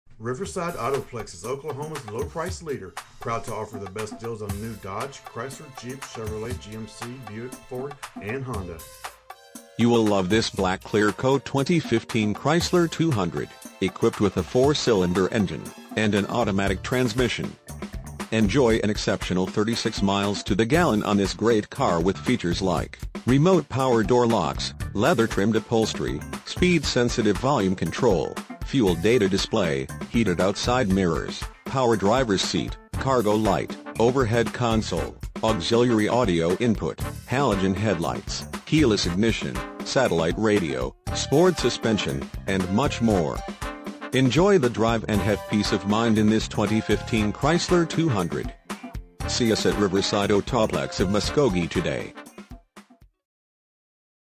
2015 Chrysler 200 Riverside Autoplex of Muskogee Muskogee, OK 74401
Chrysler Check out this Black Clear Coat Chrysler 200 4 Dr Sedan, equipped with a 4 Cylinder engine and an automatic transmission. Enjoy an exceptional 36 miles to the gallon on this great car with features like Sirius Xm Satellite Radio, Halogen Headlights, Auxiliary Audio Input - Ipod/Iphone Integration, Push-Button Start Keyless Ignition, Ambient Lighting, Electronic Stability Control, Heated Exterior Mirrors, Anti-Theft Alarm System With Engine Immobilizer, Remote Anti-Theft Alarm System, Leather-Trimmed Upholstery, 4-Way Power Adjustable Lumbar Driver Seat, Electronic Brakeforce Distribution, Front Fog Lights, Speed Sensitive Central Locking Power Door Locks, Emergency Braking Assist, Premium Cloth Upholstery, Electric Power Steering, 8 Way Power Driver Seat, and much more. Enjoy the drive and have peace of mind in this Chrysler 200. See us at Riverside Autoplex of Muskogee today!
MILES:     12
VIN:1C3CCCBB2FN529308

1711 West Shawnee
Muskogee, Oklahoma 74401